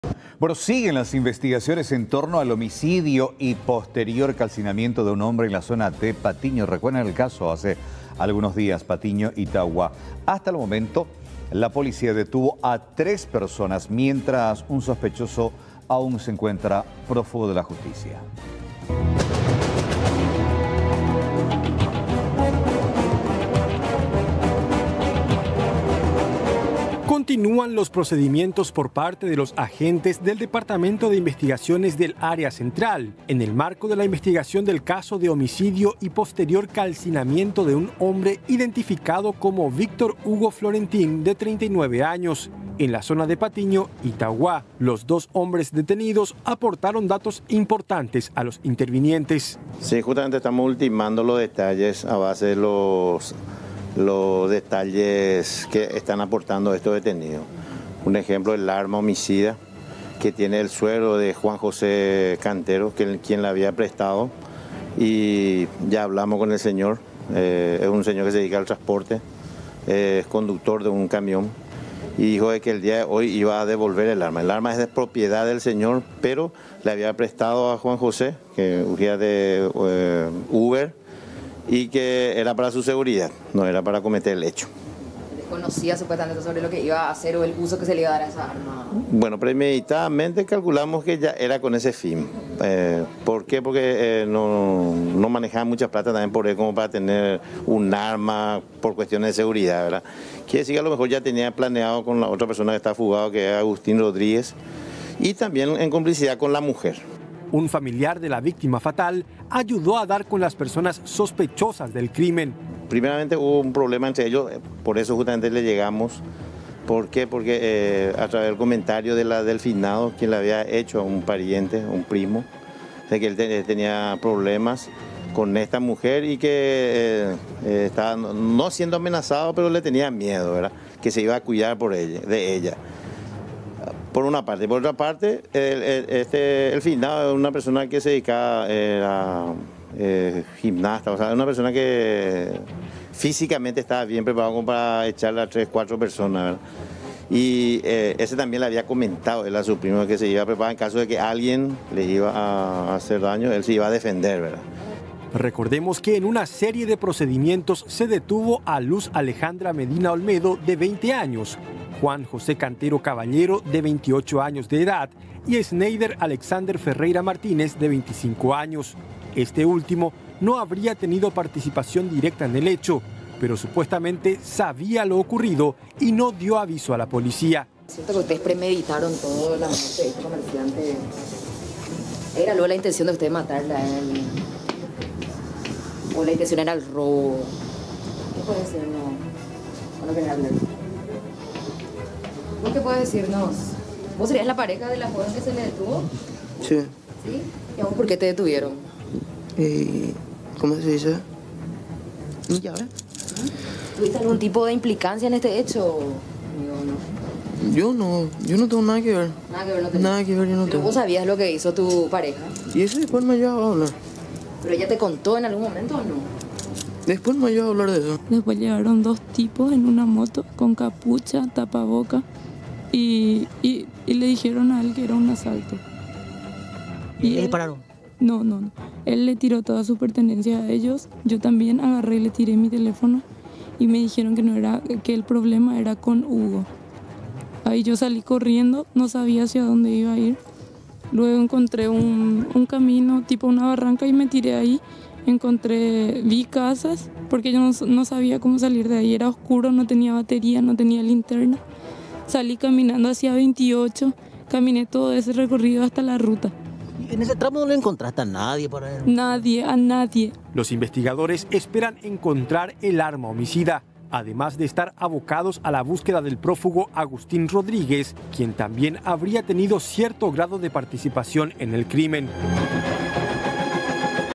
Muerto y calcinado: La investigación de la policía
El crimen ocurrió en Patiño, Areguá: Tendría un trasfondo de drogas. Un cuarto sospechoso continúa prófugo